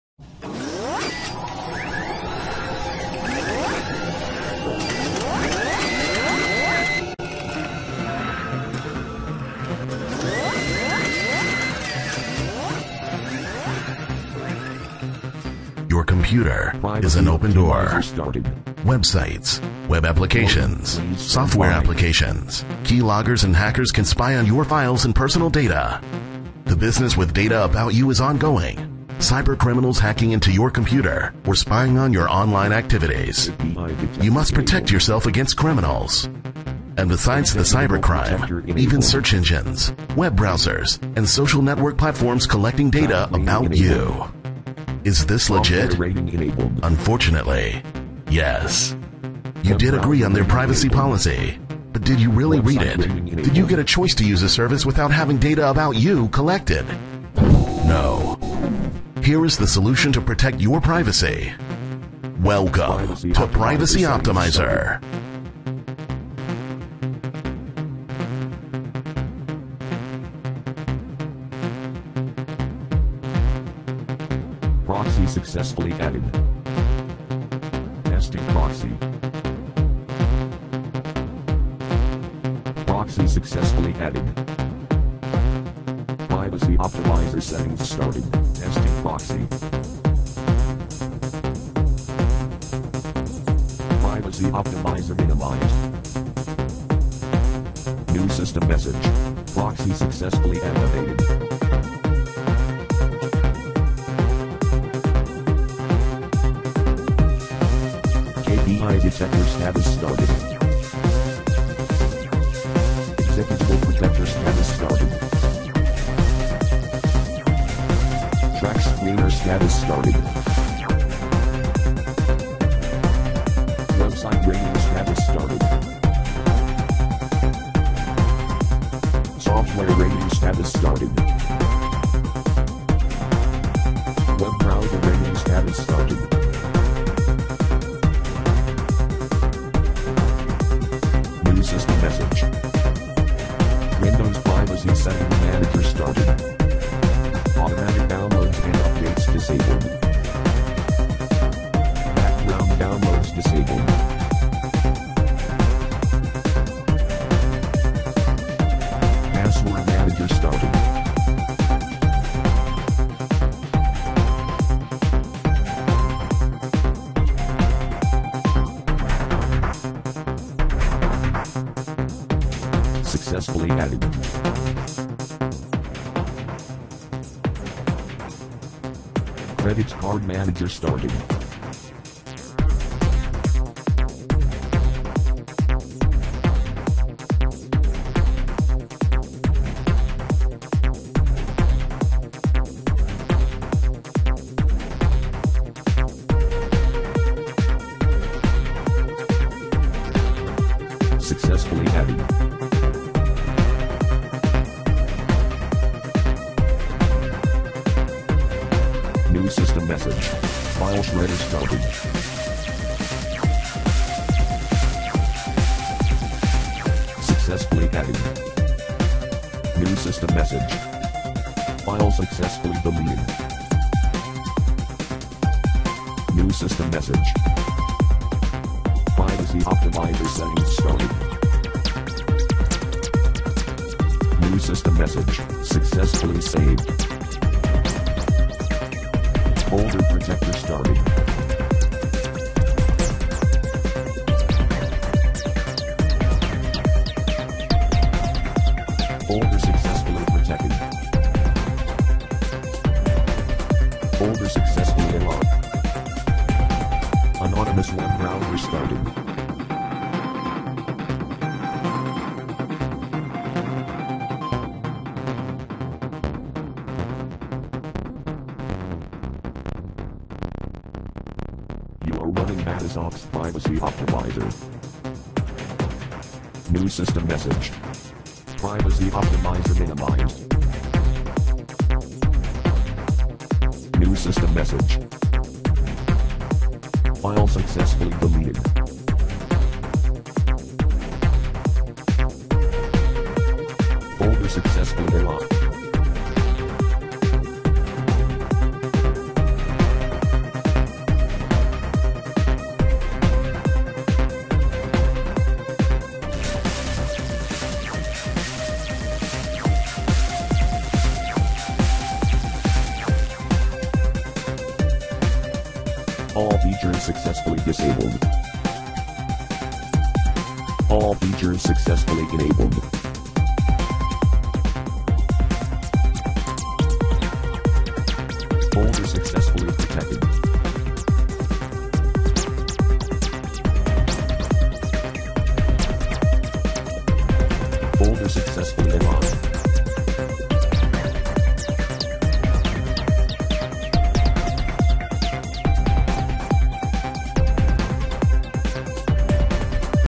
Privacy Optimizer - Make the right privacy settings!
Privacy Optimizer comes with a Tools Suite that provides 10 valuable tools to help users protect their personal information and use the Internet safely at the same time.
These tools let each individual user choose the optimal privacy settings that will keep them safe. They also serve to protect information such as files & folders, credit card numbers, passwords and more.

Users of Privacy Optimizer can choose to remain anonymous on the Internet by hiding their IP address and surfing via the included anonymous web browser. Privacy settings for Windows users can be customized and allow users to disable services they do not wish to use.

The Privacy Optimizer offers tools to rate websites, software and web browsers. Users of the program can contribute to its ratings and help each other catch any website, software or browser that is harmful to a person's computer.